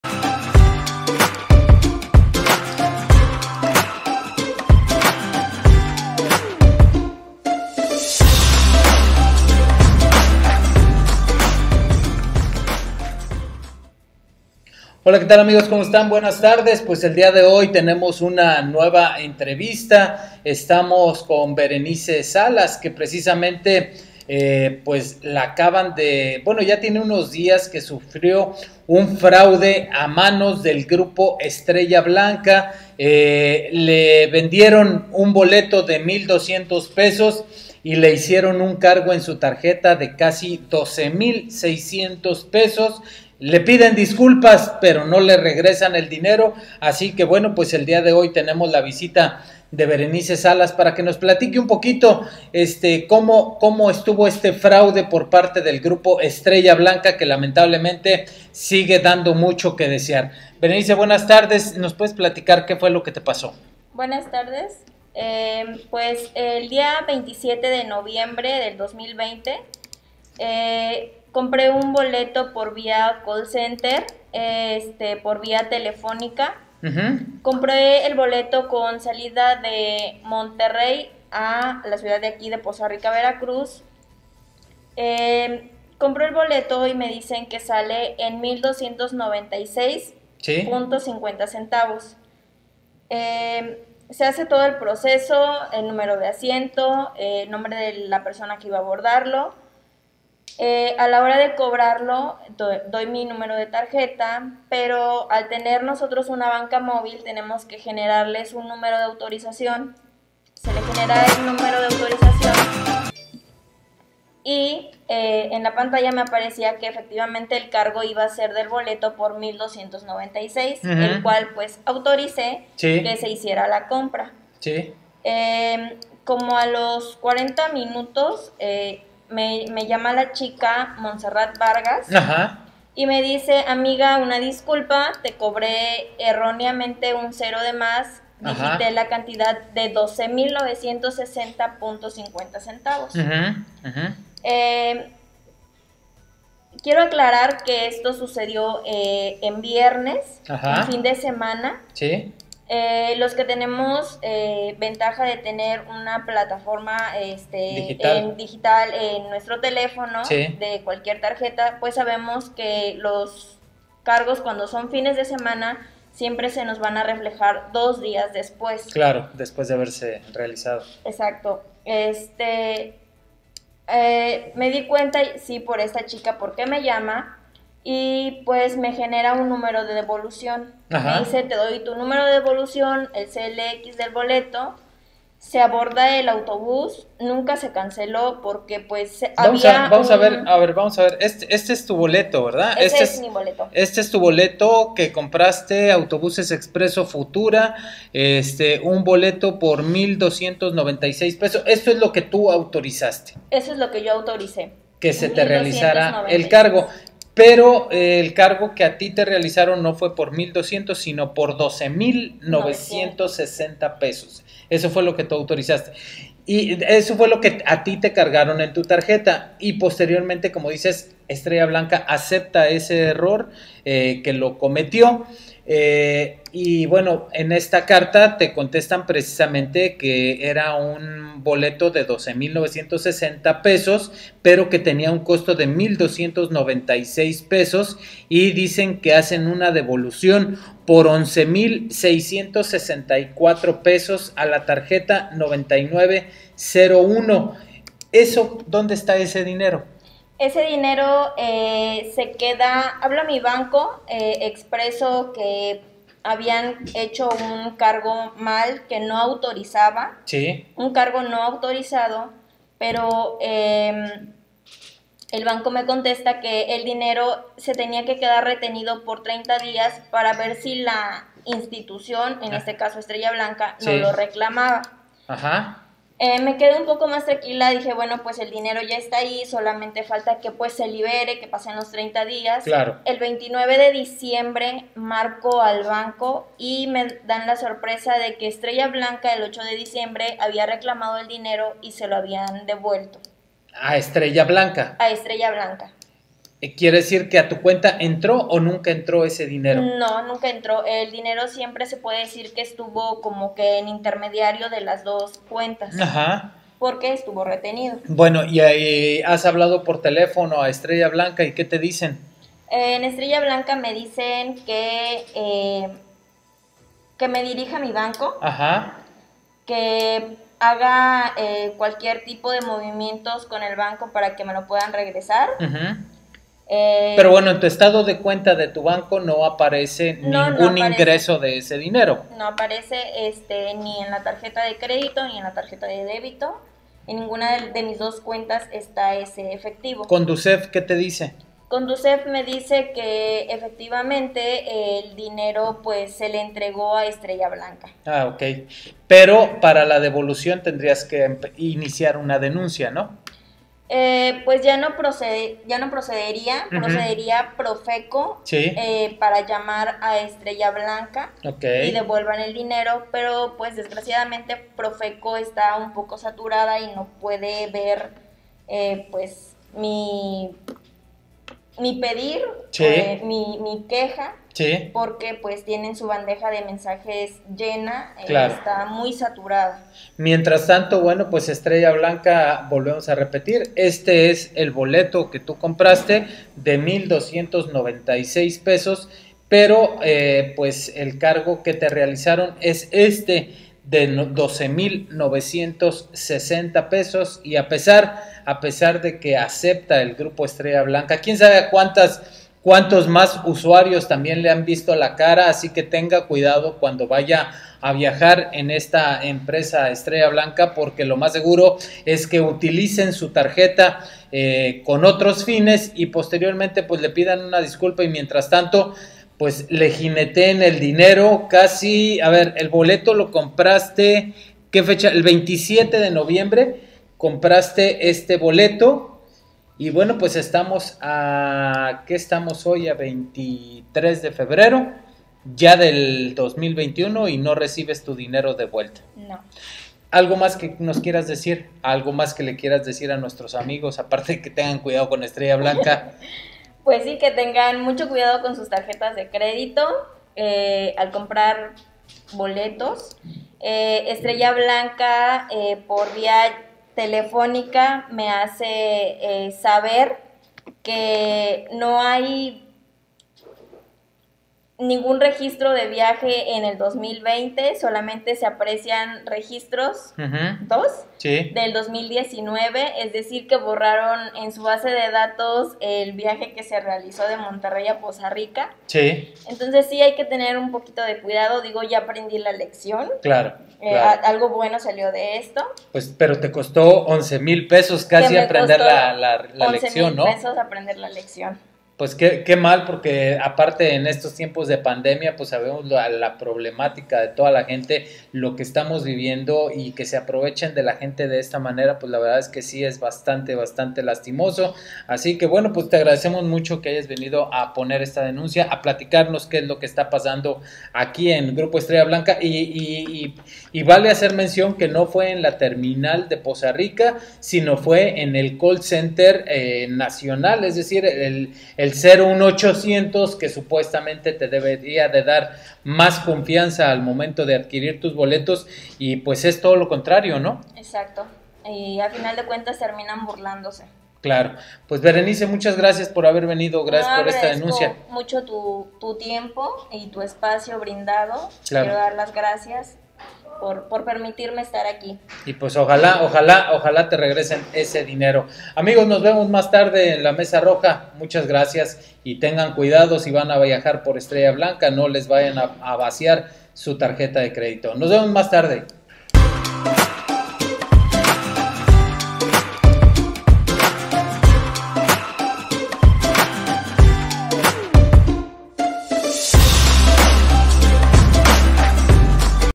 LA DEFRAUDAN EN ESTRELLA BLANCA
LA DEFRAUDAN EN @GrupoEstrellaBlancaOficial @GEB_oficial AL COBRARLE 10 VECES EL COSTO DE SU BOLETO AL VIAJAR DE MONTERREY A POZA RICA